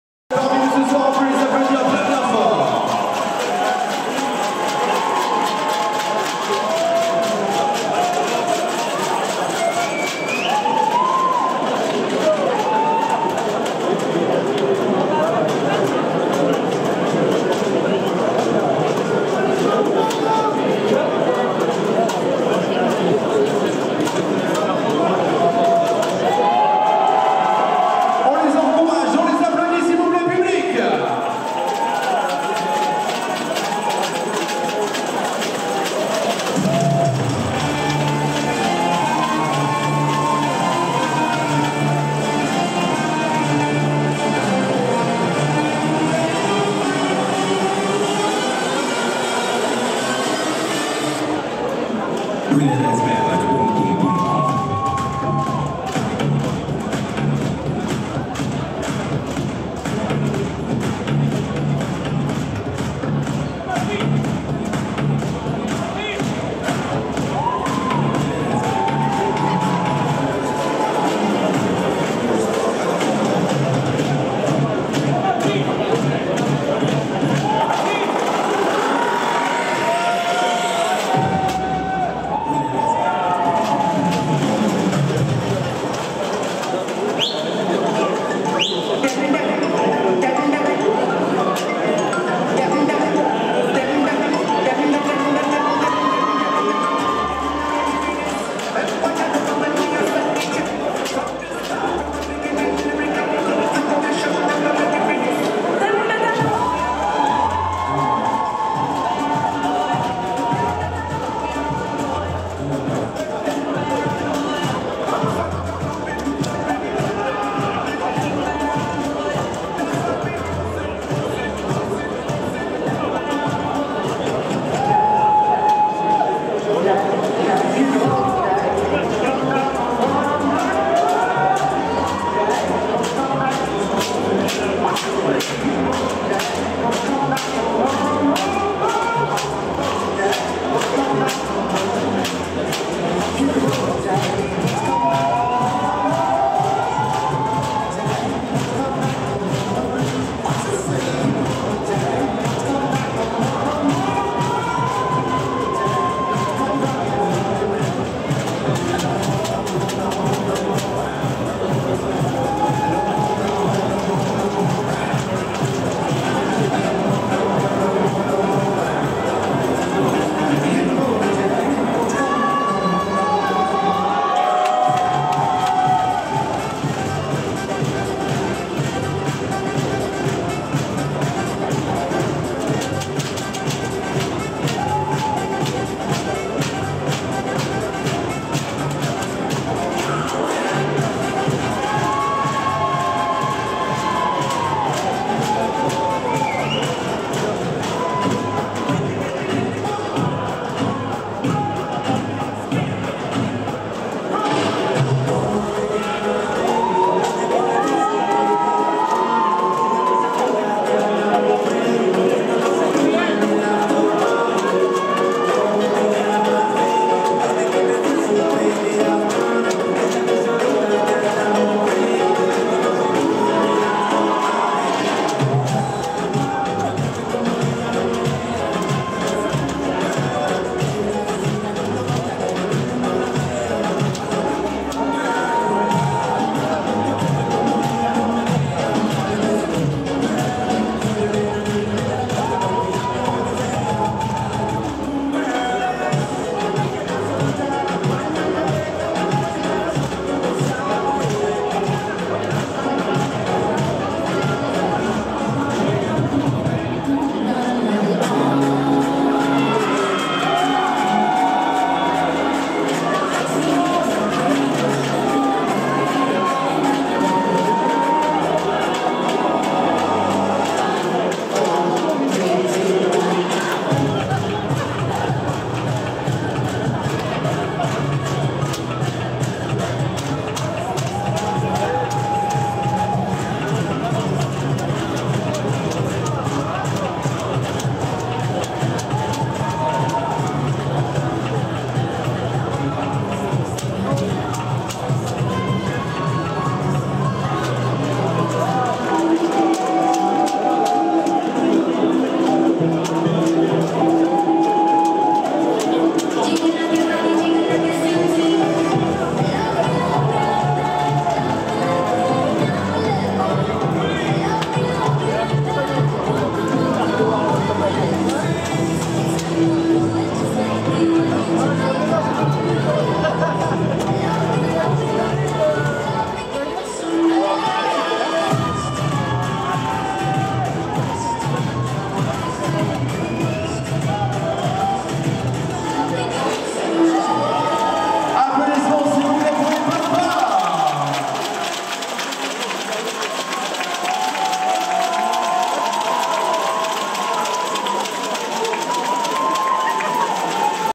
Show Basket Nanterre 92 (mi-temps) - Pompom Pharma Paris
Nanterre 92 est une équipe de Basket ProA dont le coach Pascal Donnadieu est entraîneur adjoint de l'équipe de France.
Nous intervenons pour certains matchs de Nanterre 92 lors desquels nous animons, encourageons et présentons un show en mi temps, quarts temps ou encore lors des temps morts.
Cette fois-ci il s'agissait d'une mi temps !